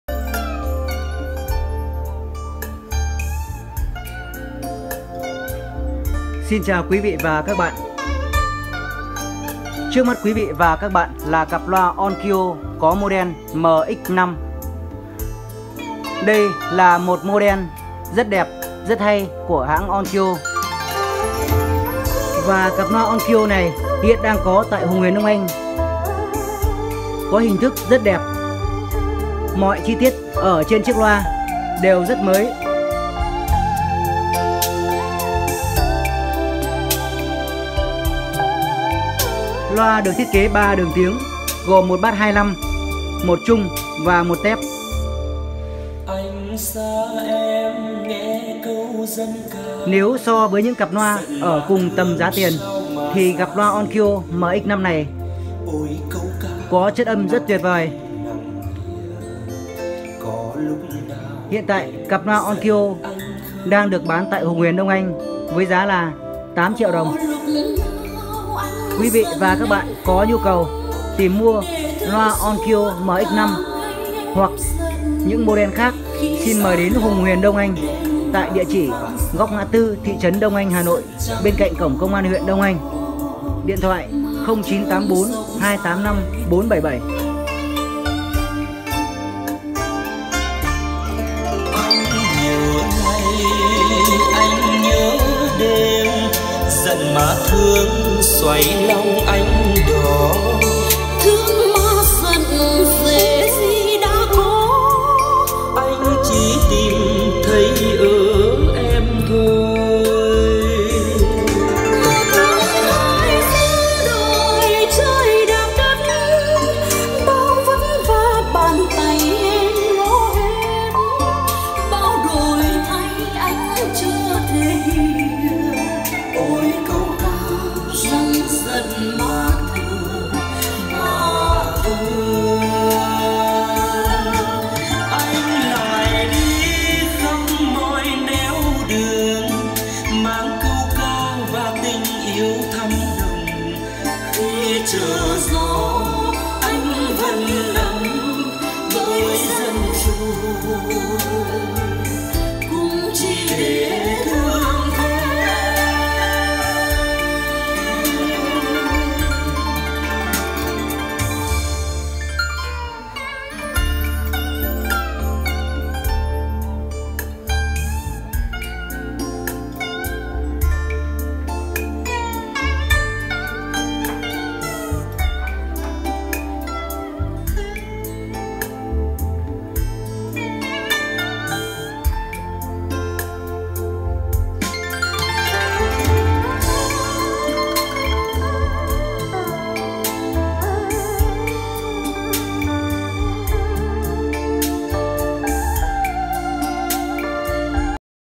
Bé nhỏ nhưng tràn đầy năng lượng - loa Onkyo DX-5 tại HÙNG HUYỀN ĐÔNG ANH
Loa Nhật bát 25- Onkyo DX-5 đẹp suất sắc.
    Giá : 8 tr
Lh : Hùng Huyền Đông Anh  - Góc ngã tư thị trấn Đông Anh Hà Nội